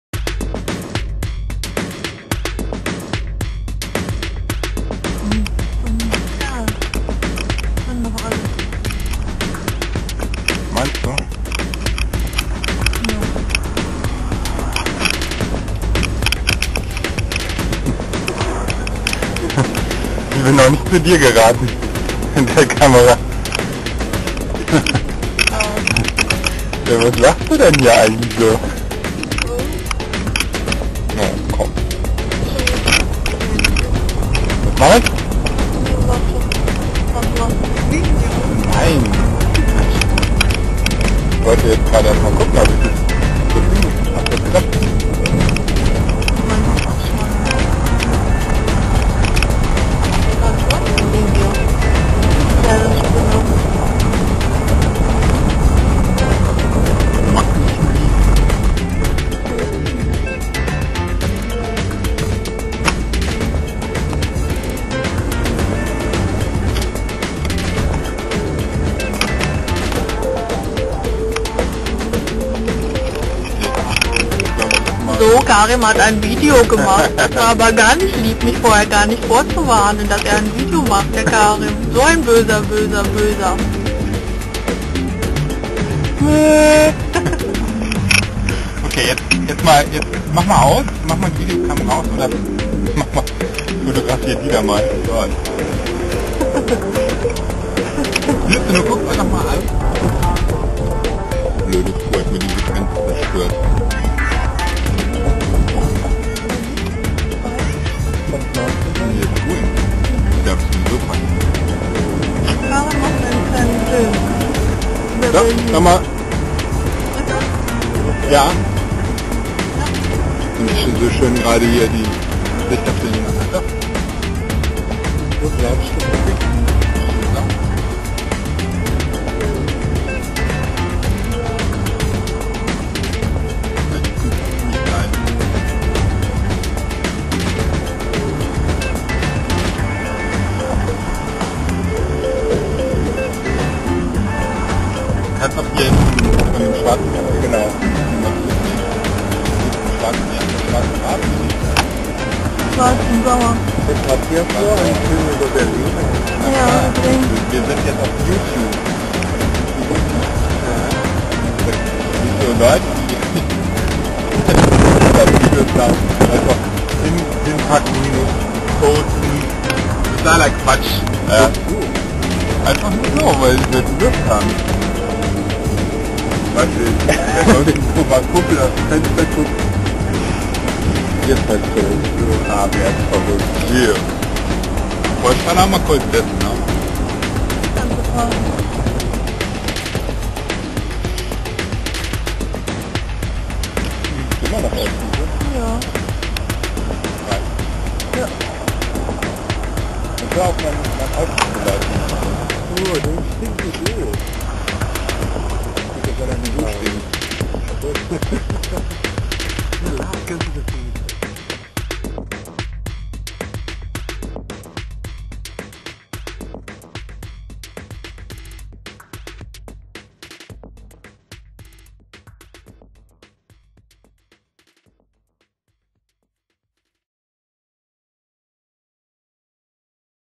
sundance kids part 1
this is berlin: went to the fleamarket at arkona platz last sunday. agnes, bunftel (working name for our son hihi) and gil were sitting in our good old renault espace, driving through prenzlauer berg,kastanienallee to get to the fleamarket. watch part 1. by the way: i did the music myself...